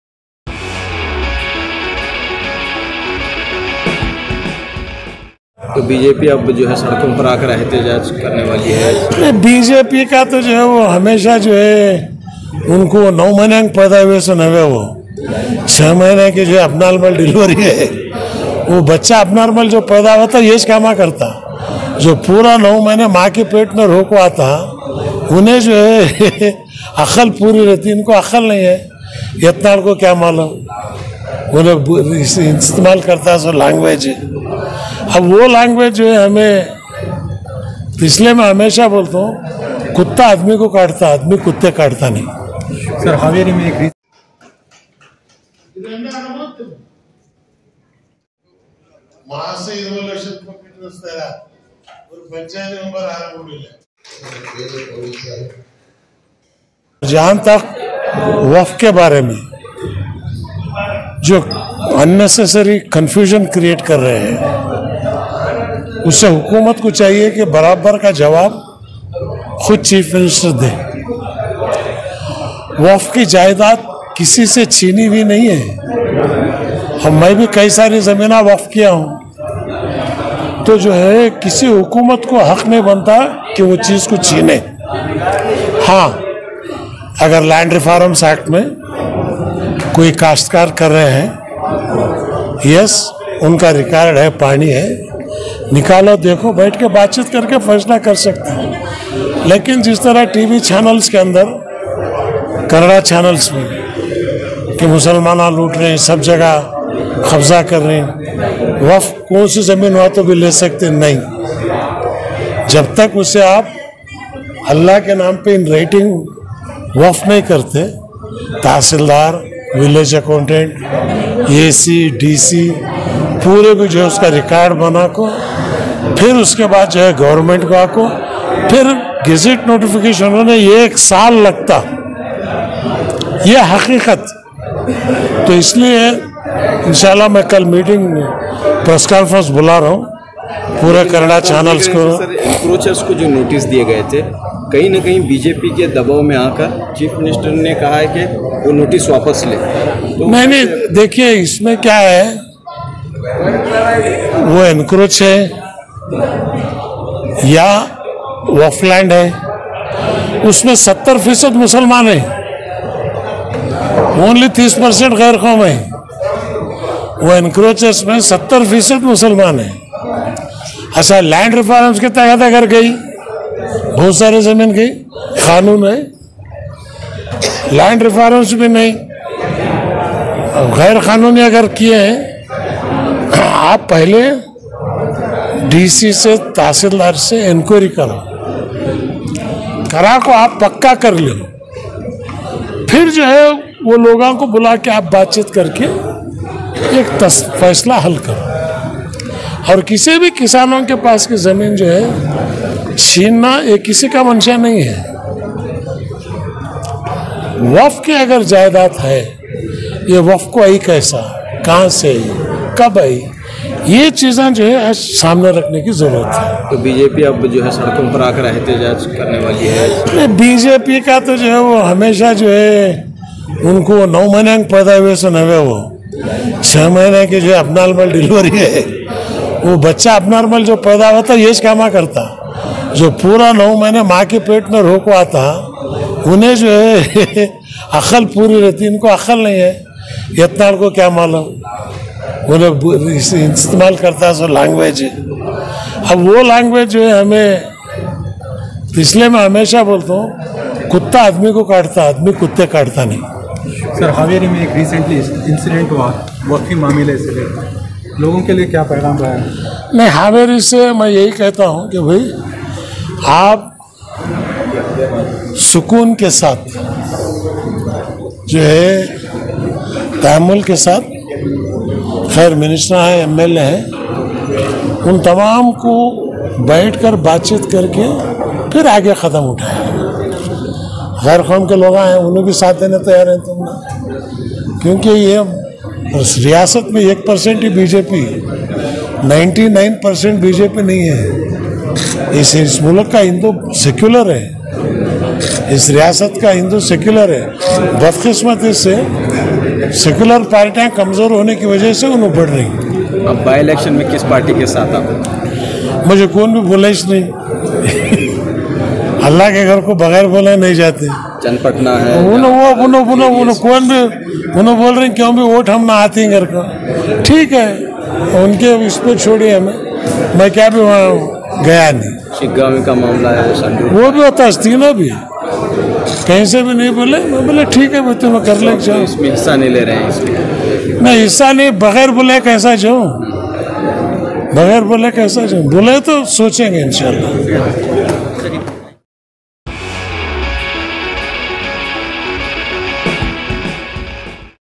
CM Ibrahim strongly criticises BJP over Wakf issue | Speaks about By elections saying no party invit
وقف پر سیاست، سابق مرکزی وزیر سی ایم ابراہیم نے BJP کو تنقید کا نشانہ بنایا